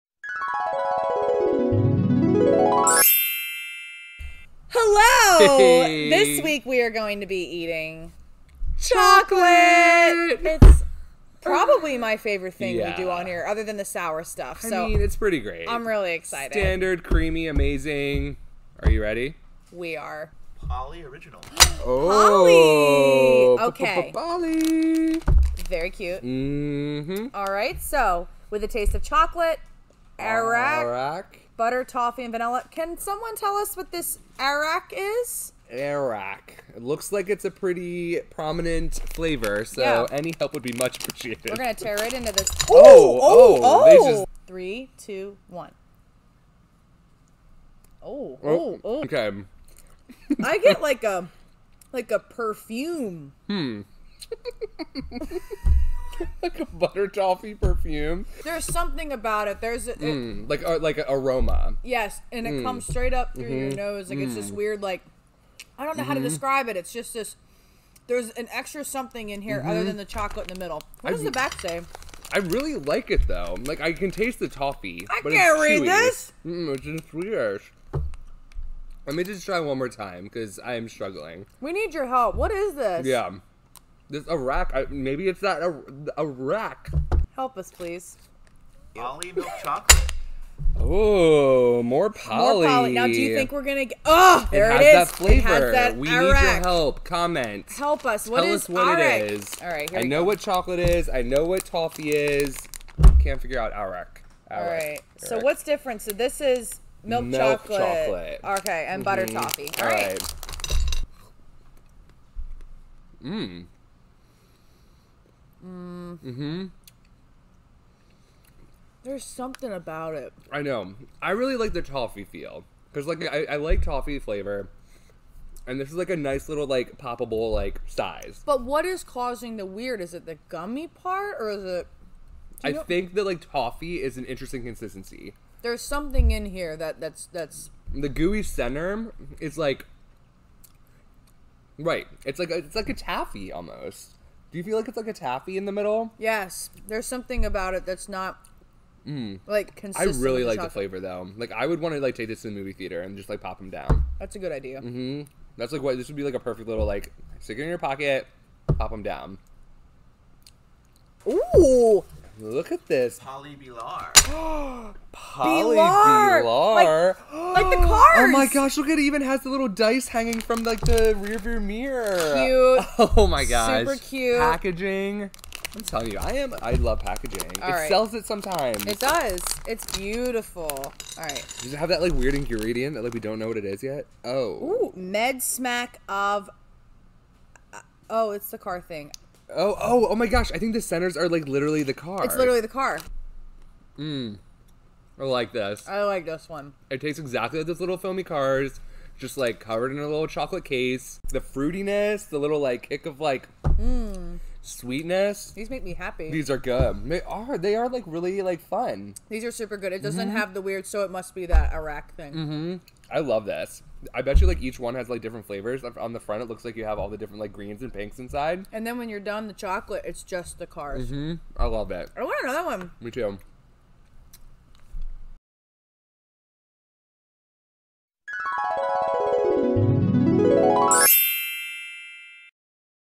Americans Try Cloetta Polly - Part One
Gabby & Tony give Cloetta Polly a try!

PRODUCTS MENTIONED-